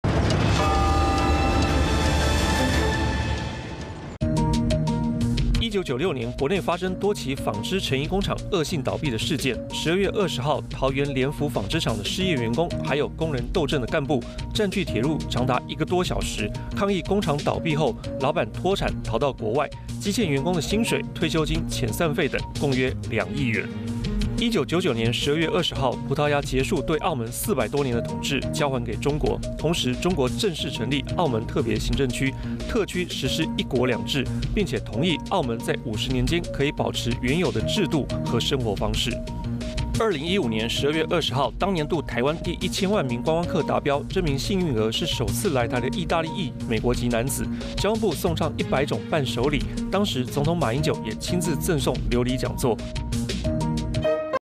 【歷史上的今天】聯福紡織惡性倒閉 員工佔據鐵路抗議｜華視新聞 20211220
12月20日帶您回顧歷史上的今天，1996年12月20日「桃園聯福紡織廠」員工佔據鐵路抗議，工廠惡性倒閉後，老闆欠員工的薪水、退休金及資遺費等共約2億元；1999年12月20日，葡萄牙將澳門統治權交還給中國，結束對澳門400多年的統治；2015年12月20日台灣累計千萬名觀光客達標，幸運兒是首次來台的義大利裔美國男子。